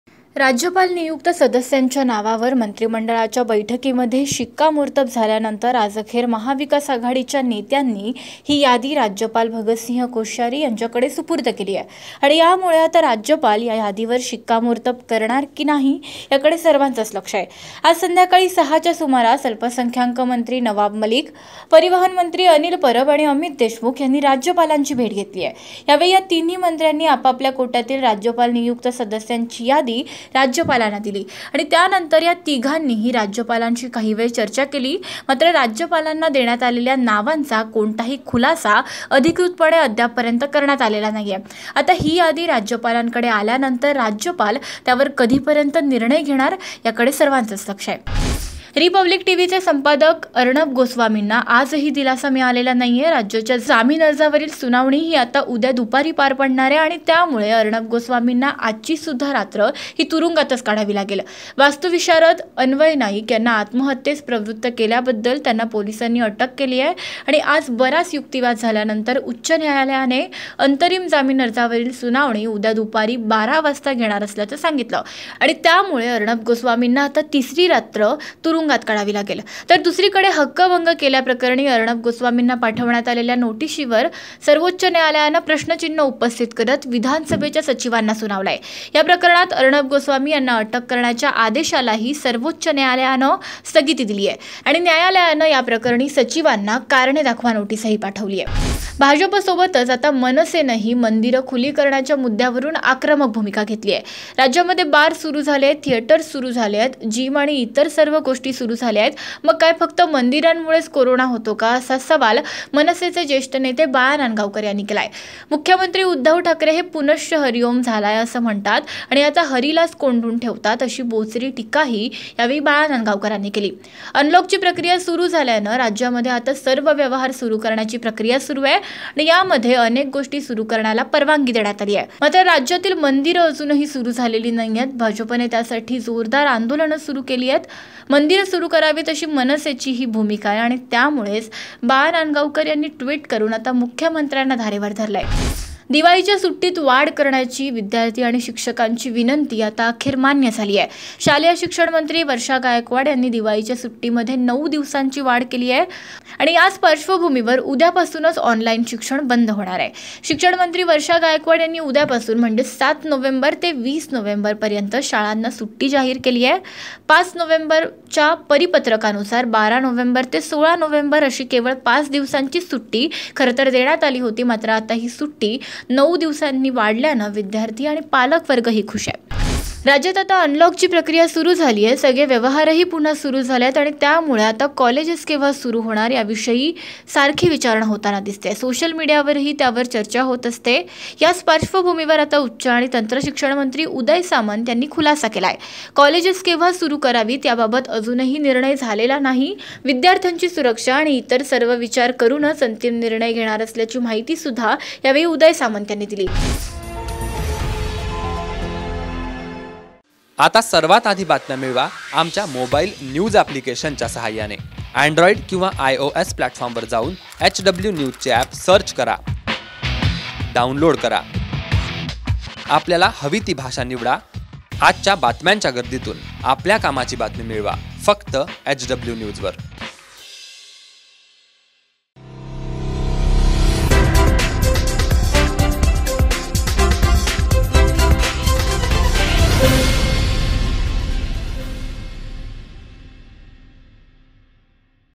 महाविकासआघाडीच्या मंत्र्यांकडून अखेर 'ती' यादी राज्यपालांकडे सुपूर्द, निर्णयाकडे राज्याचे लक्ष
राज्यपाल नियुक्त सदस्यांच्या नावावर मंत्रिमंडळाच्या बैठकीत शिक्कामोर्तब केल्यानंतर आज अखेर महाविकास आघाडीच्या नेत्यांनी ही यादी राज्यपाल भगतसिंह कोश्यारी यांना सादर केली. त्यामुळे राज्यपाल या यादीवर शिक्कामोर्तब करणार की नाही? याकडे सर्वांचं लक्ष लागलं आहे. आज अल्पसंख्याक मंत्री नवाब मलिक, परिवनह मंत्री अनिल परब आणि अमित देशमुक यांनी राज्यपालांची भेट घेतली. यावेळी या तिन्ही मंत्र्यांनी आपआपल्या कोट्यातील राज्यपाल नियुक्त सदस्यांची यादी राज्यपालांना दिली. त्यानंतर या तिघांनीही राज्यपालांशी काहीवेळ चर्चा केली. मात्र, राज्यपालांना देण्यात आलेल्या नावांचा कोणताही खुलासा करण्यात आलेला नाही. आता ही यादी राज्यपालांकडे आल्यानंतर राज्यपाल त्यावर कधीपर्यंत निर्णय घेणार? याकडे सर्वांचे लक्ष लागले आहे.